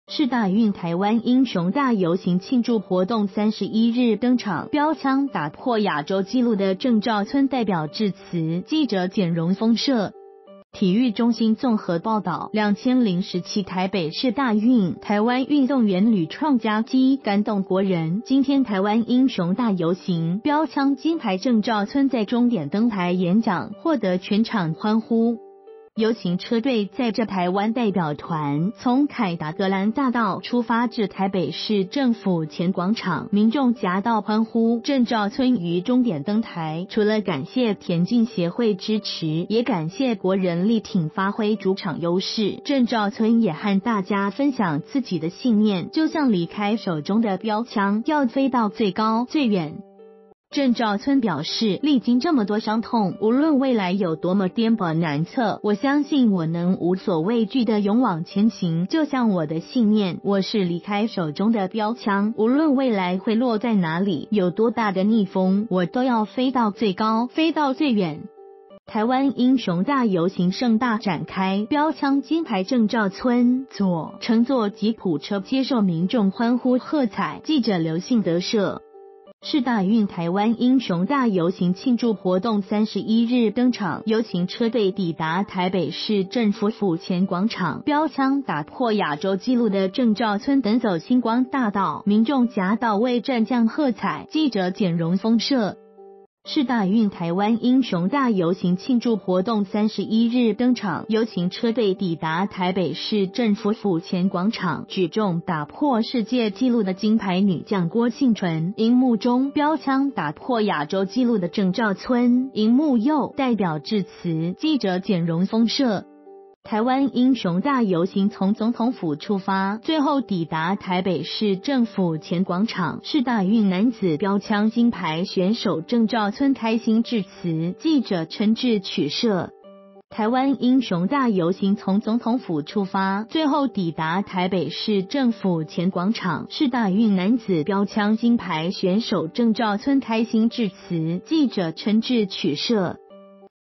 「像標槍飛到最高最遠」 鄭兆村演說有洋蔥
世大運「台灣英雄大遊行」慶祝活動31日登場，標槍打破亞洲紀錄的鄭兆村代表致詞。（記者簡榮豐攝）

發表評論評論如下。